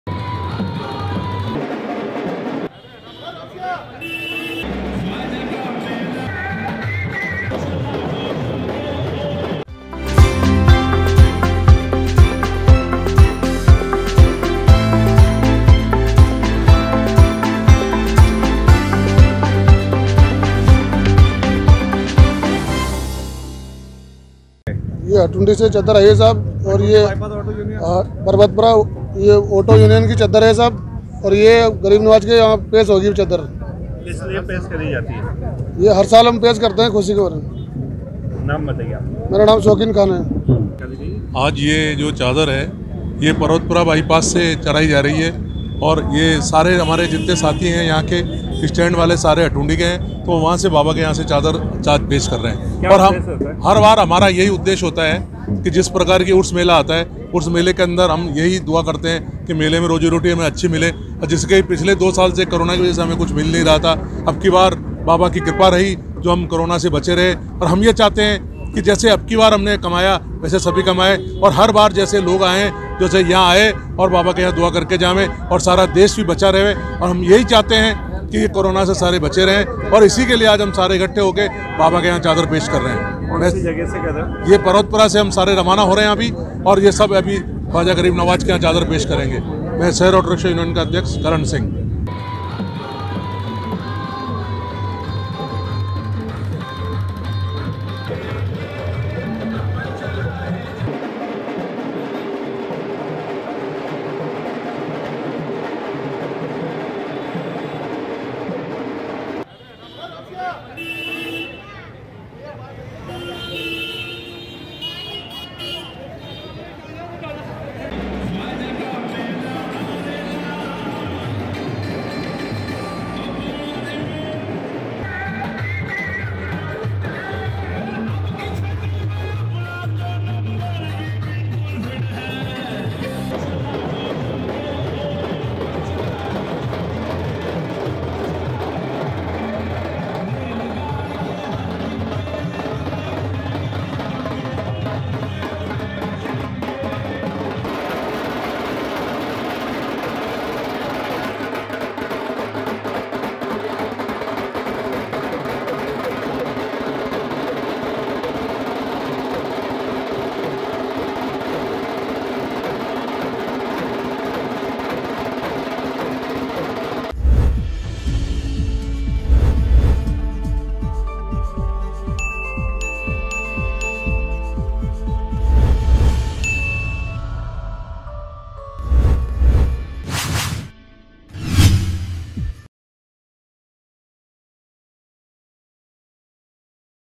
Ajmer | अजमेर टेंपो यूनियन ने ख्वाजा के दरबार में चढ़ाई चादर | MTTV INDIA
अजमेर टेंपो यूनियन ने ख्वाजा के दरबार में चढ़ाई चादर
उर्स के पर्व पर अच्छे रोजगार के बाद ख्वाजा के दरबार में चढ़ाई चादर